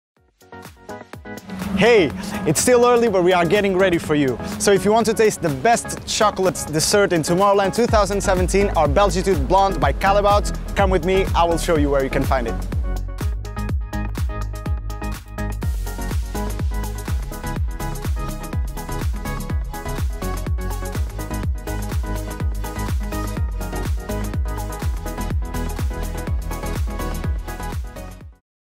Callebaut Belgitude Chocolate Bar @ Tomorrowland 2017 - Where to find us?
Belgitude is back! Come visit us at @Tomorrowland for sky-high fireworks, mind-blowing music and finely crafted chocolate desserts.